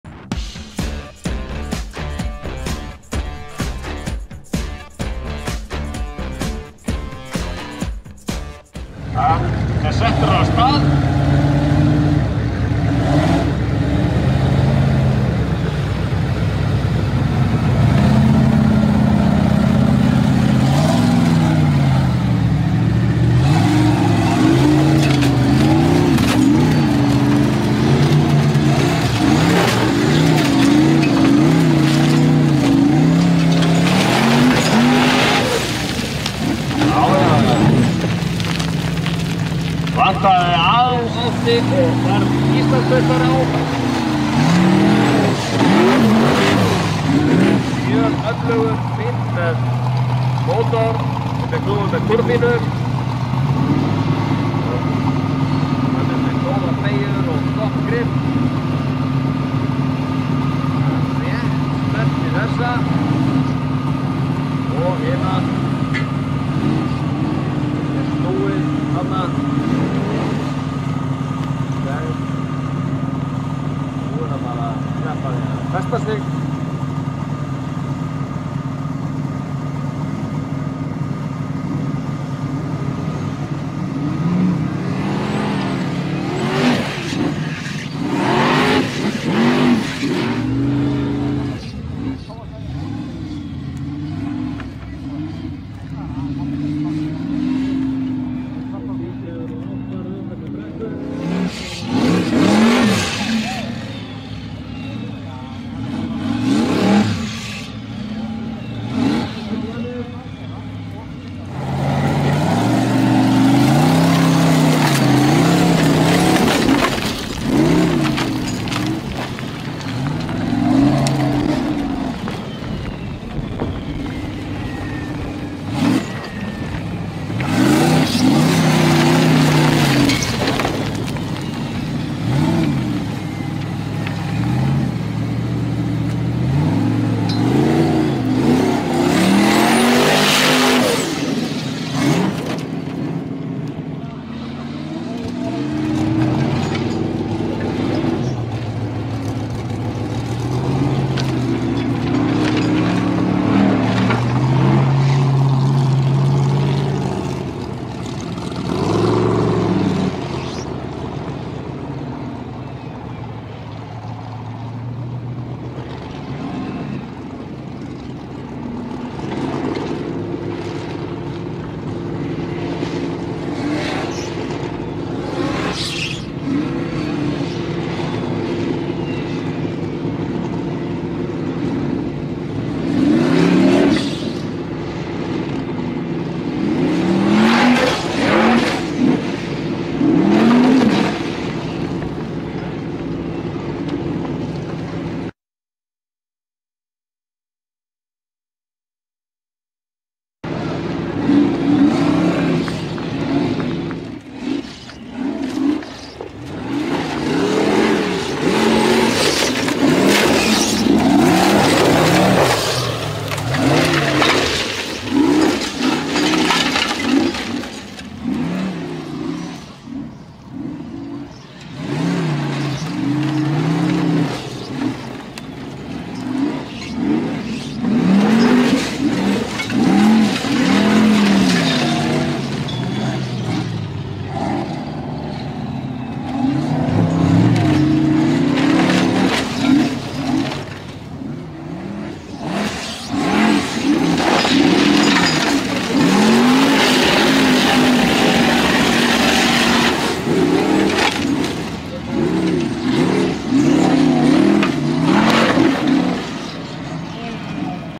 Spade Racing driver Þórður Atli track 1 - 6 , 4 jun Isavia torfæran Formula offroad Iceland
Formula offroad 2023 Formula offroad 2023 Photo: Heiða Björg Jónasdóttir
torfæran 2022